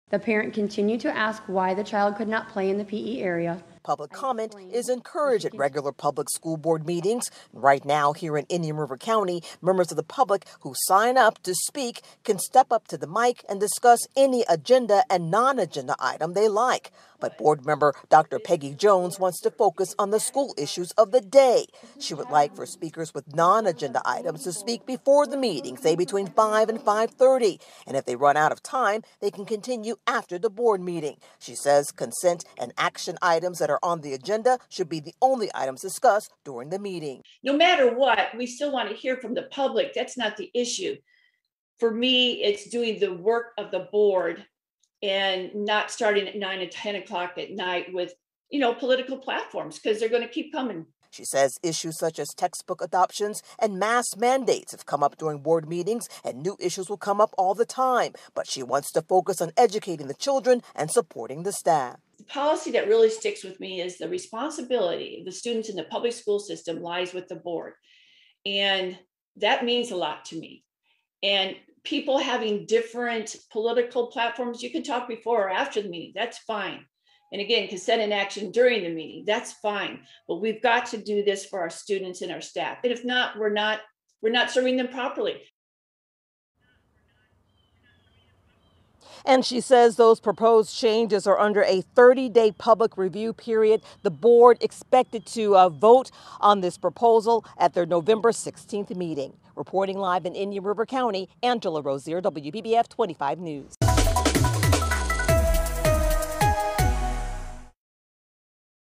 Changes to the public speaking policy in IRC school board meetings
WPBF's Angela Rozier has the details.